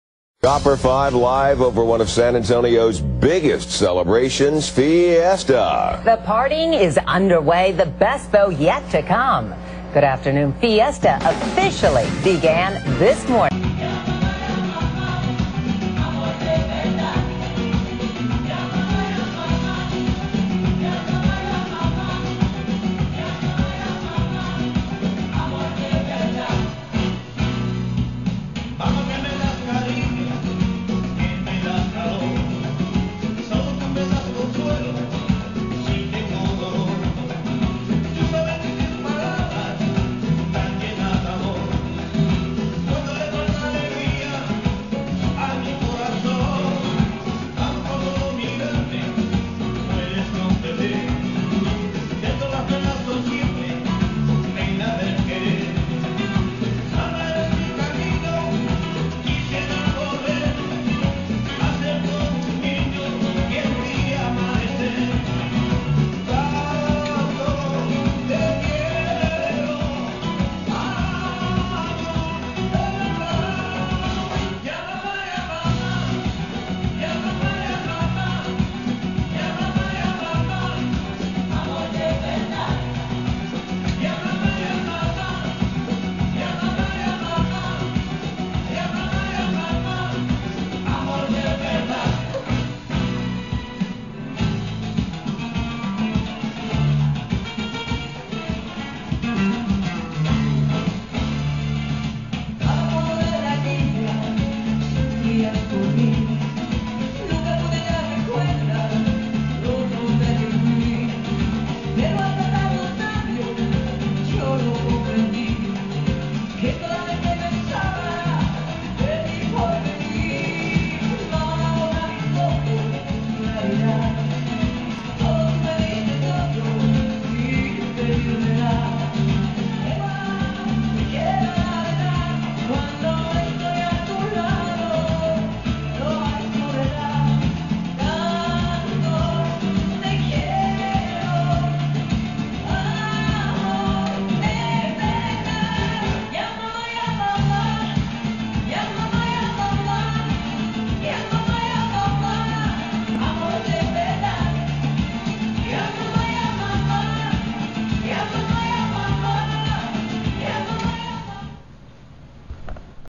Viva Fiesta San Antonio !
Roland from The Springtime65 show kicks off Fiiesta in His own way
saying Love who you our and Let's P A R T Y  !

Rememebr not to " Drink and Drive" .

This video was made for The Springtime65 show
Saturdays 10:30pm on TWC 20Digital and 19 Grande Cable
and 99 AT&T U-Verse for The SA PATV